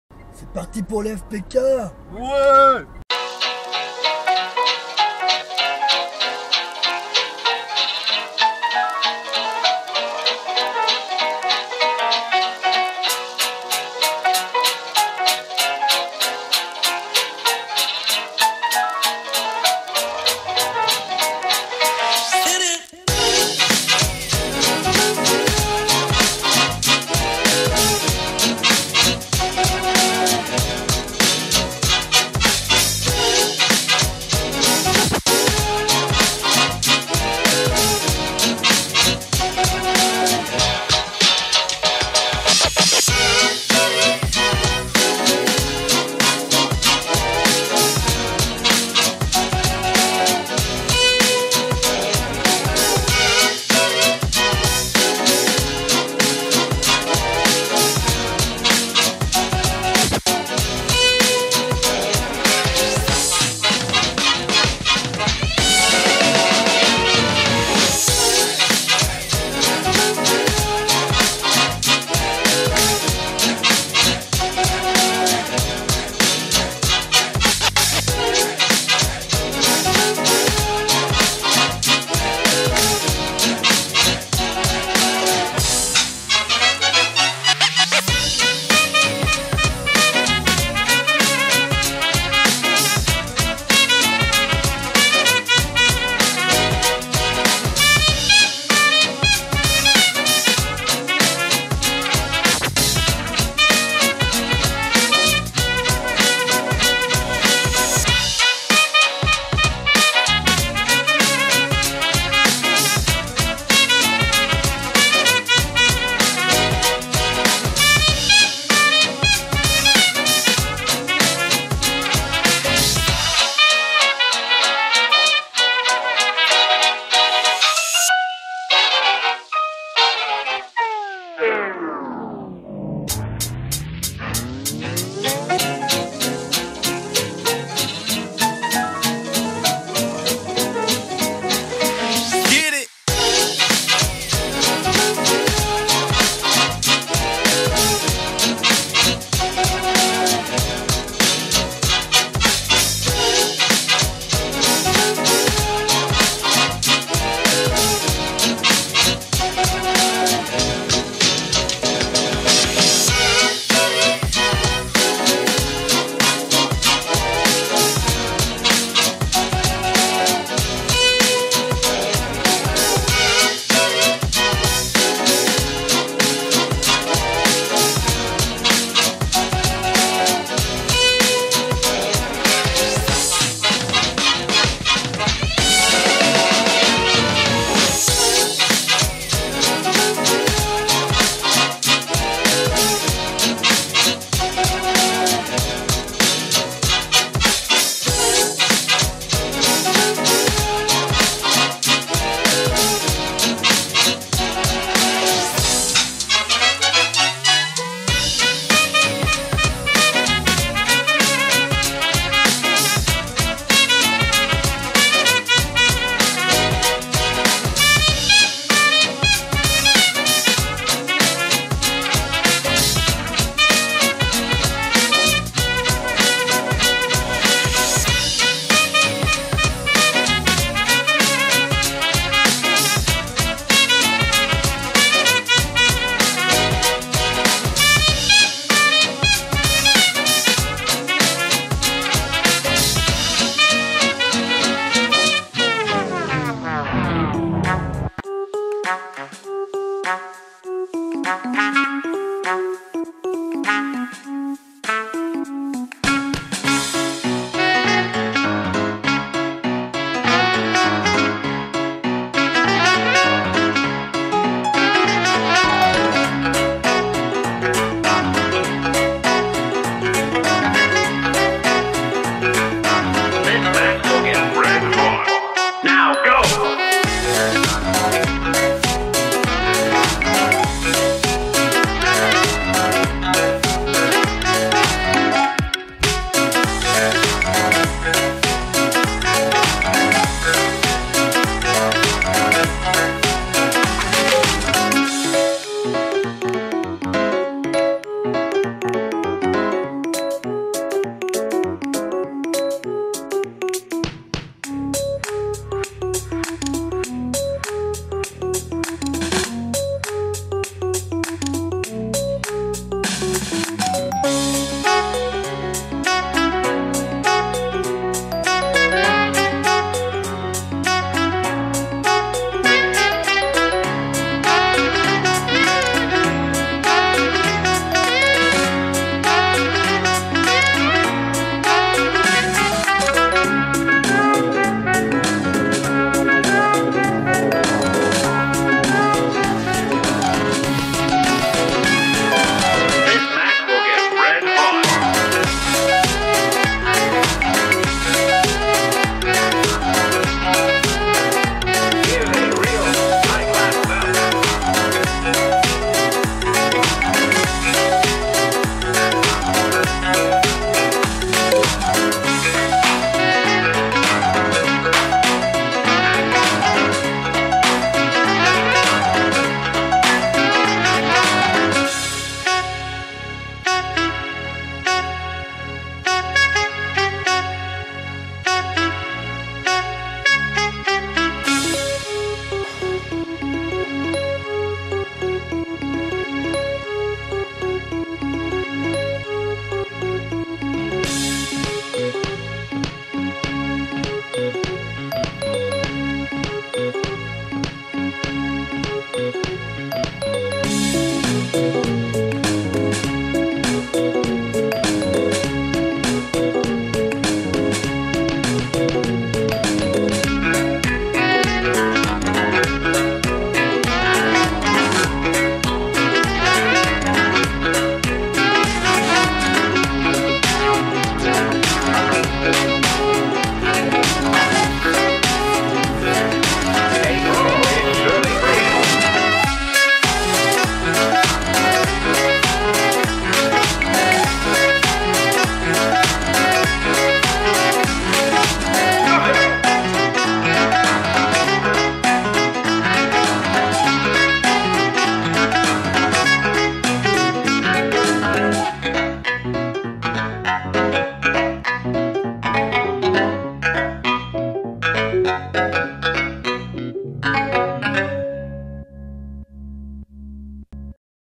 FPK - Reims 2018
''Petite'' vidéo du week-end FPK 2018 à Reims. Je vais faire deux parties parce que mine de rien, on en prend de la vidéo avec un tout petit quadri qui vol 5 à 6 minutes :D